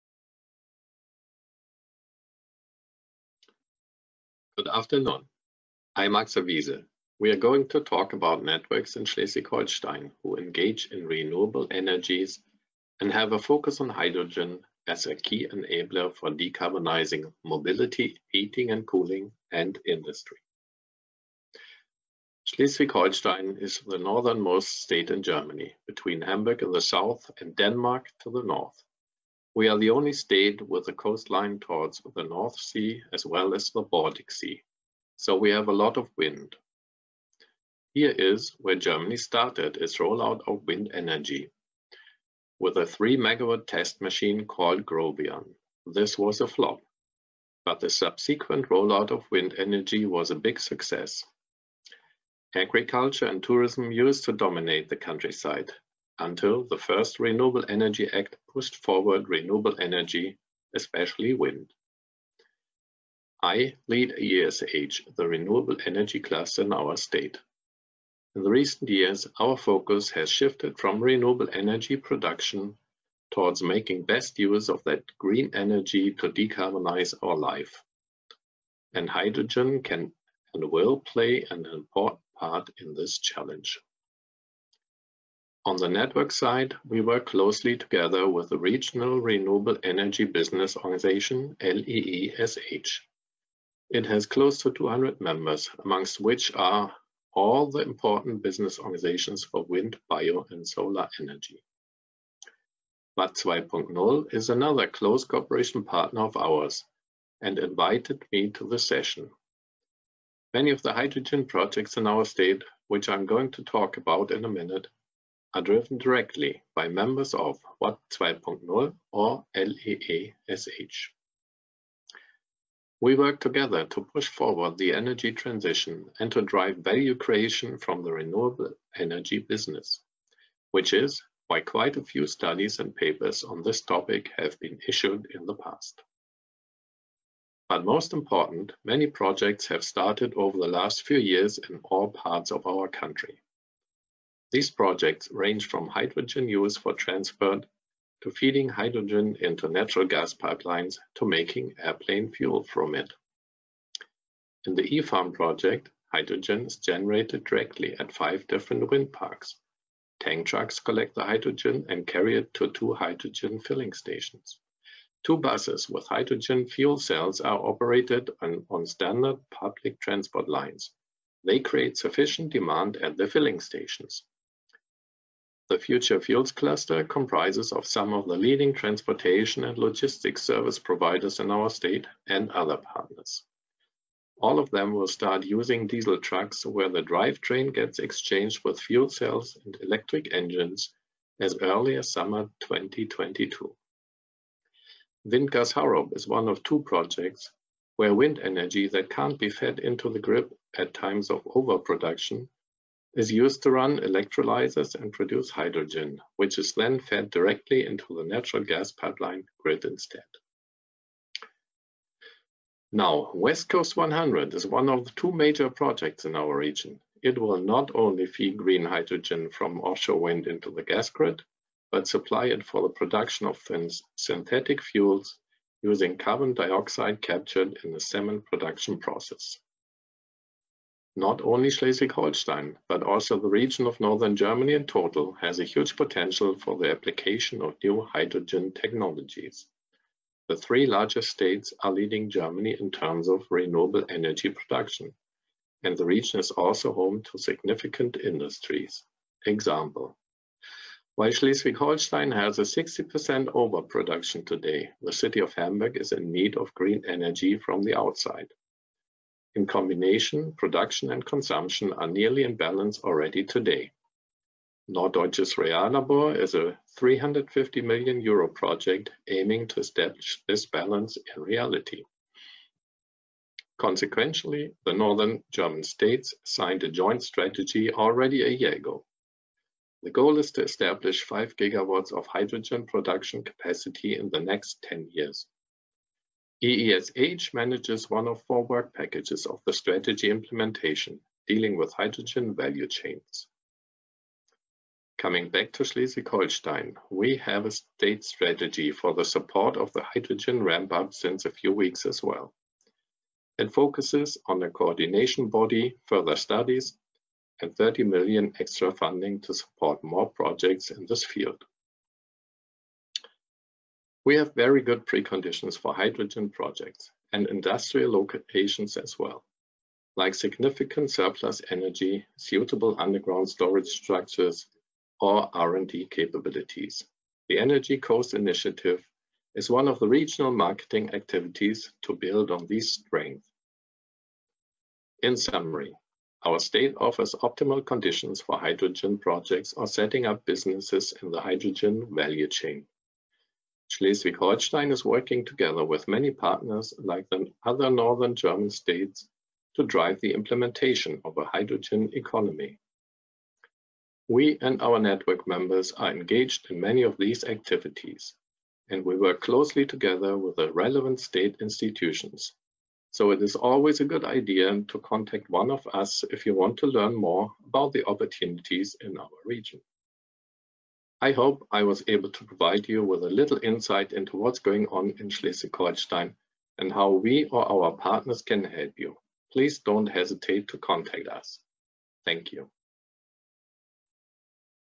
H2 networks in Schleswig-Holstein - "Watt2.0 Hydrogen Forum - "Wind Energy Hamburg" (2.12.2020)
H2 networks in Schleswig-Holstein - presentation in pecha-kucha format, held in December 2020 on the "Watt2.0 Hydrogen Forum" as part of the "Wind Energy Hamburg" digital event.